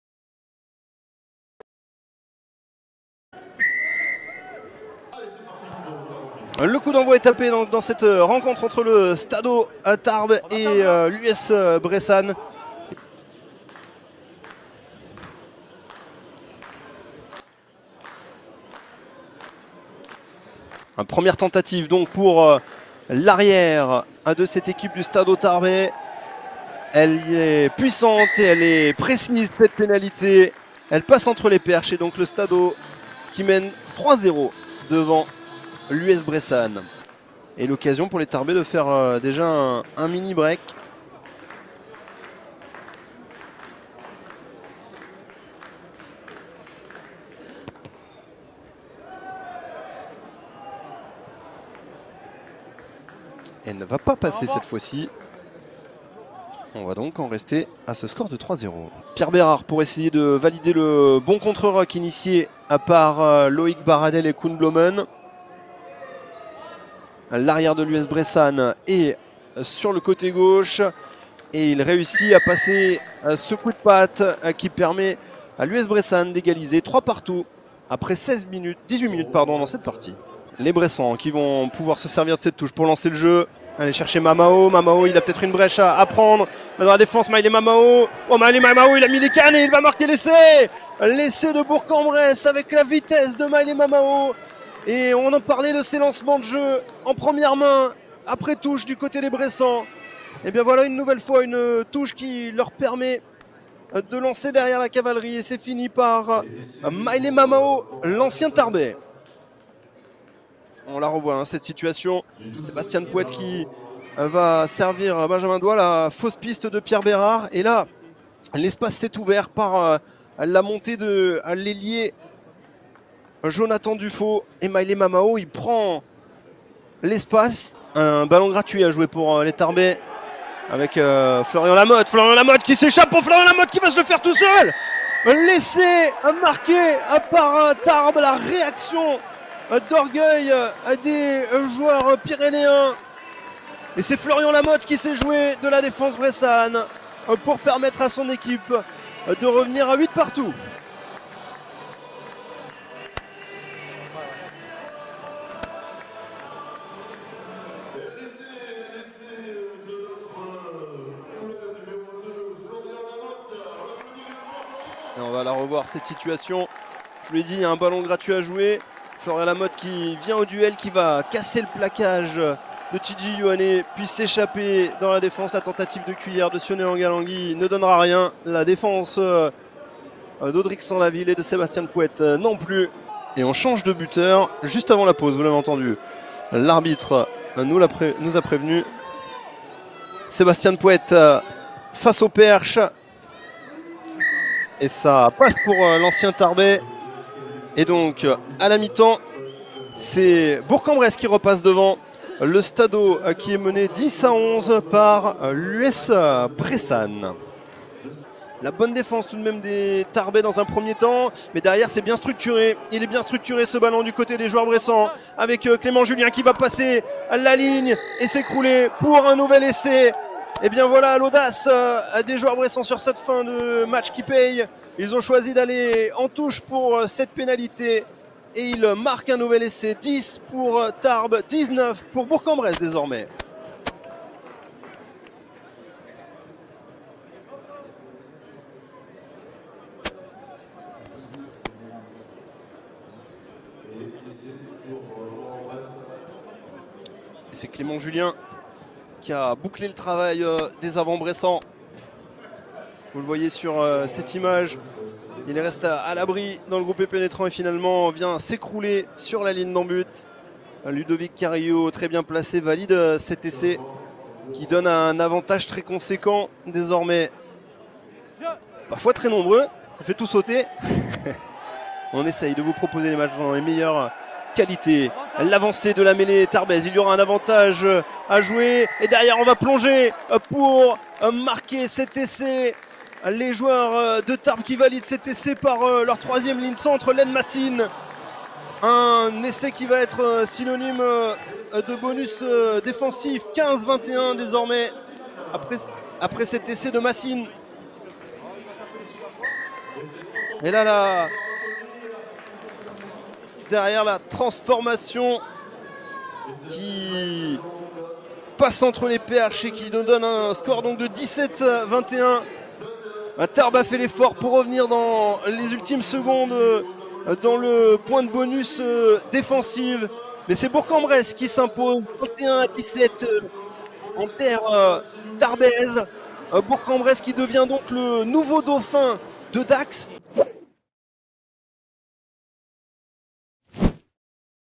Nationale J13 - Stado Tarbes vs USBPA Rugby
Retrouvez les meilleurs moments de la rencontre comptant pour la J13 de Rugby Nationale entre le Stado Tarbes et l'USBPA.

► Accédez en exclusivité au meilleur du Rugby sur RUGBYZONE.TV :

Site internet : rugbyzone.tv
Application mobile : IOS / Google play